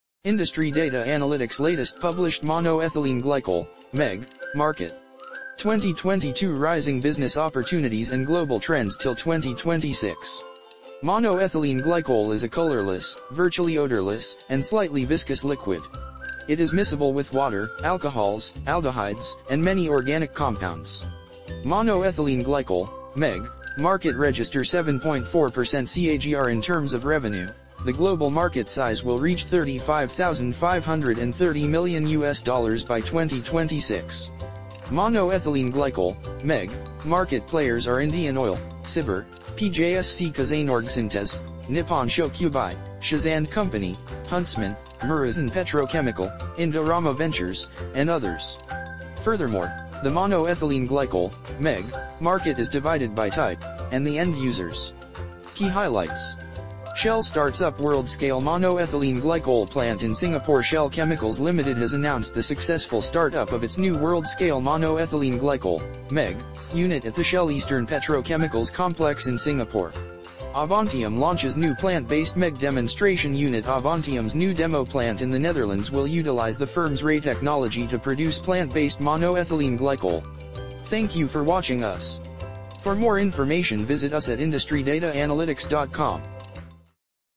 Monoethylene Glycol (MEG) Market | Industry Data Analytics | IDA
Industry Data Analytics Latest Published Monoethylene Glycol (MEG) Market Research Report - Growth, Trends And Forecast (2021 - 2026) By Types, By Application, By Regions and By Key Players: SABIC, Sinopec, Formosa Plastics Group (FPG), Shell, and Others.

Global Monoethylene Glycol (MEG) market size (most likely outcome) will be a year-over-year revenue growth rate of XX% in 2021, from US$ 26740 million in 2020. Over the next five years the Monoethylene Glycol (MEG) market will register a 7.4% CAGR in terms of revenue, the global market size will reach US$ 35530 million by 2026. Monoethylene glycol is a colorless, virtually odorless and slightly viscous liquid. It is miscible with water, alcohols, aldehydes and many organic compounds.